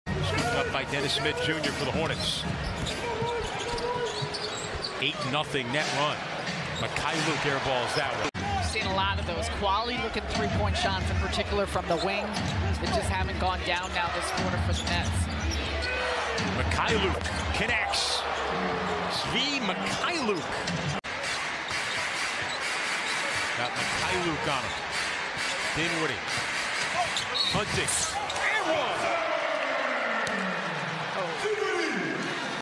Svi Mykhailiuk : All Possessions (2023-03-05)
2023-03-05 Brooklyn Nets vs Charlotte Hornets: Player Highlights Svi Mykhailiuk
    3 PTS, 0 REB, 0 AST, 0 STL.
        1 of 2 from 3.
        1 of 2 FGs in 9:34 minutes.